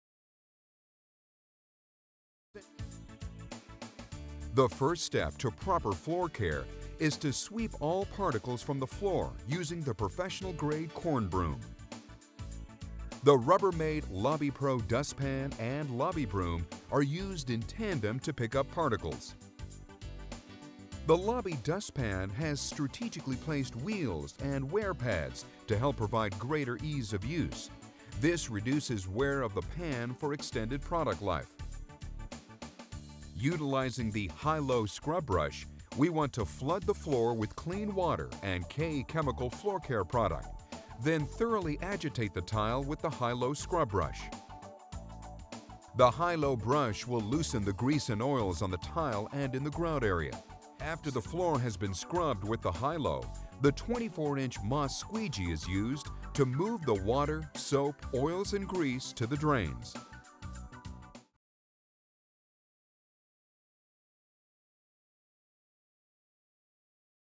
Get those floors clean
Proper floor cleaning showcasing Century Product LLC's corn broom, lobby broom and dust pan, hi lo scrub brush and 24" floor squeegee